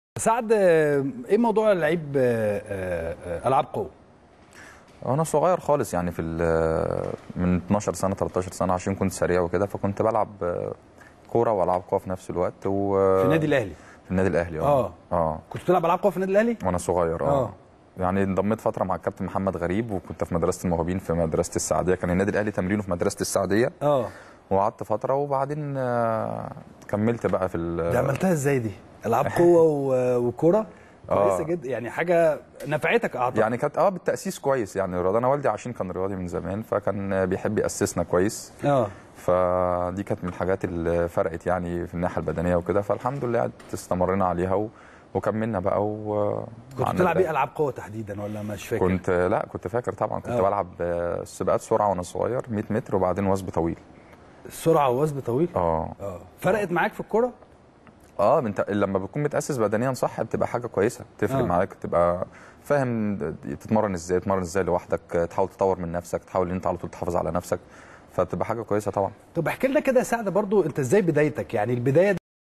سعد سمير : انا كنت لاعب العاب قوى قبل ما اصبح لاعب كرة قدم
التردد: 12341 أفقي.
القمر: نايل سات.
معامل الخطأ: 5/6.
معدل الترميز: 27500